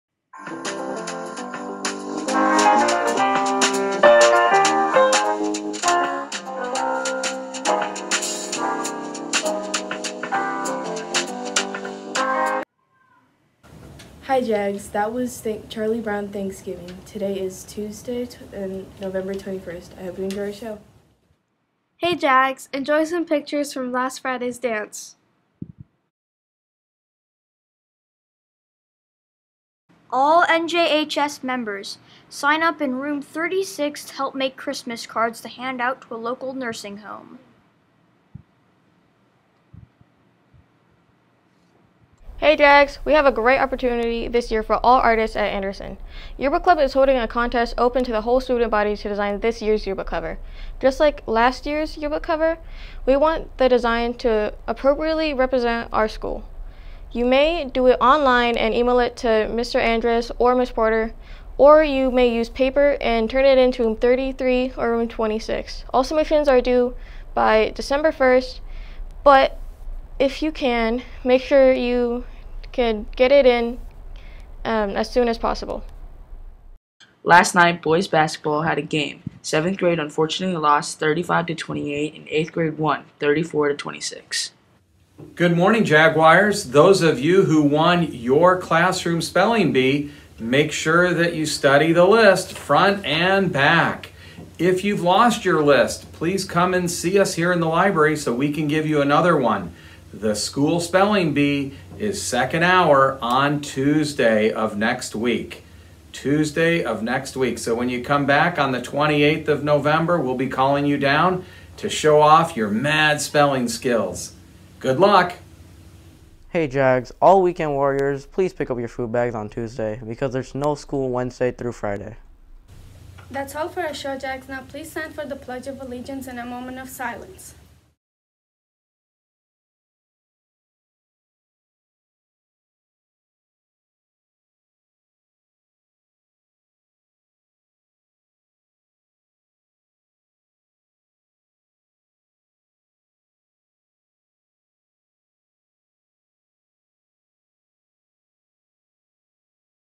KJAG - November 21, 2023
Basketball game results, School Spelling Bee reminder, dance pictures, etc.